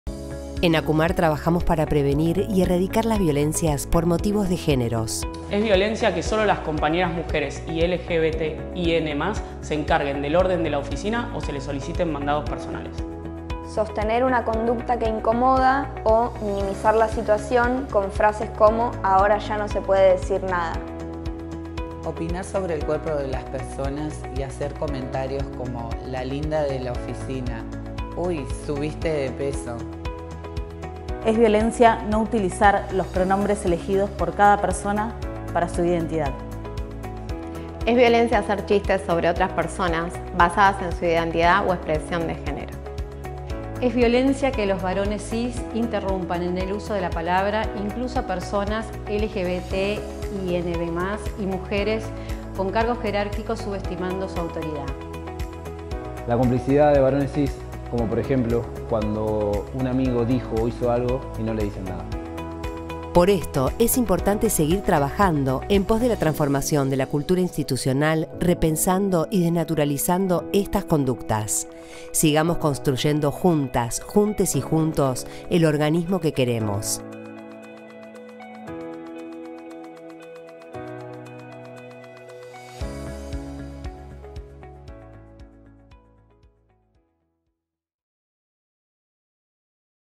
25 de noviembre: Día Internacional de lucha contra las violencias por motivos de géneros
25 de noviembre: Día Internacional de lucha contra las violencias por motivos de géneros

En ACUMAR trabajamos para prevenir y erradicar las violencias por motivos
de géneros.

En el ámbito laboral estas violencias se reproducen cuando, por ejemplo, los tratos, roles y tareas son asignados según la identidad de género, marcando diferencias entre varones y mujeres cis, lesbianas, gays, bisexuales, travestis, trans, intersexuales, no binaries y otras identidades o expresiones de género.

Sigamos construyendo juntxs la sociedad y el organismo que queremos.

Prevenir, recomponer y mejorar es la tarea.